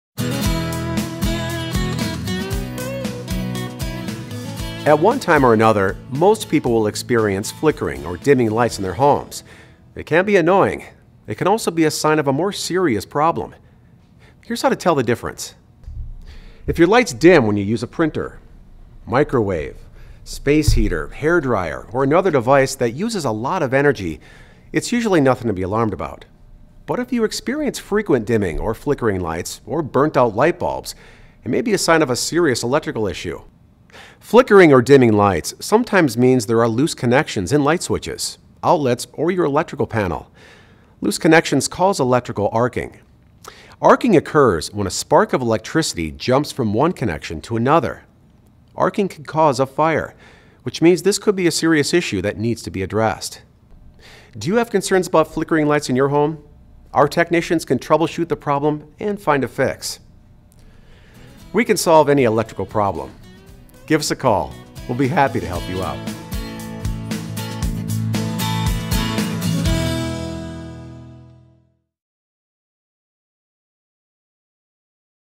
Quick Tips Video – The Solution For Flickering Lights
Lights that randomly flicker or dim is a sign that something in your electrical system needs to be addressed.  Watch this video to learn more.